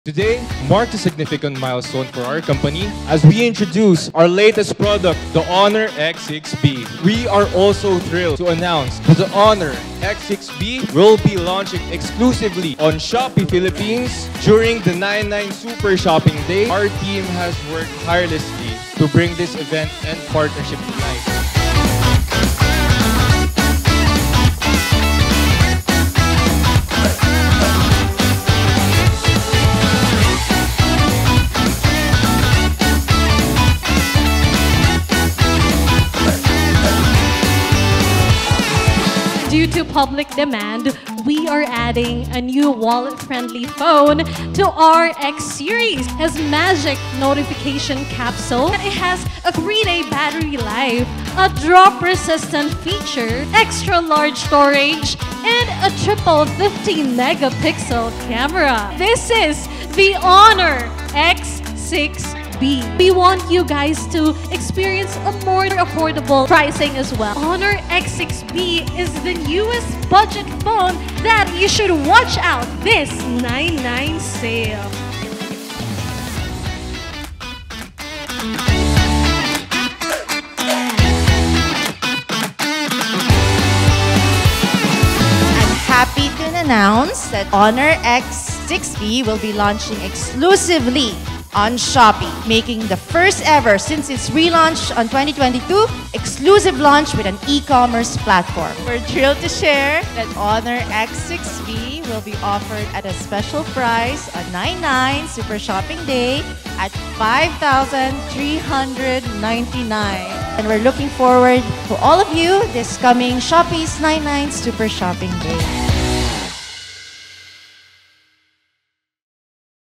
Grand Launch of HONOR X6B
Thrilled to announce that HONORX6b is launching exclusively on @shopee_ph this 9.9 Super Shopping Day! 🤩

Want to know what are the capabilities of your budget-friendly phone? Watch until the end to see how DURABLE this phone is. 😉

Also, catch the PISO SALE this 9.9 at exactly 12AM via HONOR PH's Facebook and Shopee social accounts.

ADD TO CART NOW!